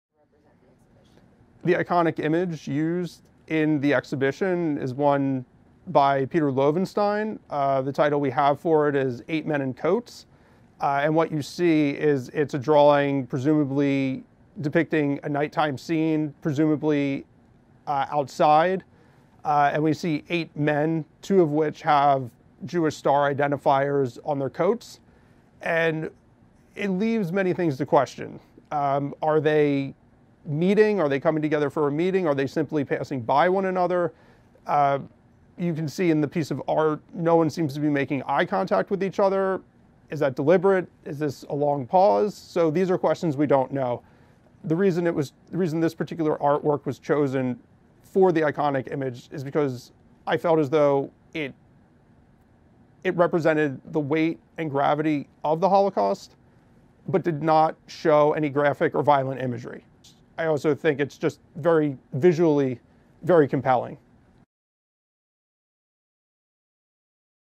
Choosing the Iconic Image for "Rendering Witness: Holocaust-Era Art as Testimony"
Peter Loewenstein's drawing "Eight Men in Coats with Stars" is the iconic image used for the Museum's exhibition "Rendering Witness: Holocaust-Era Art as Testimony." The exhibition curator explains the choice of this drawing in this video.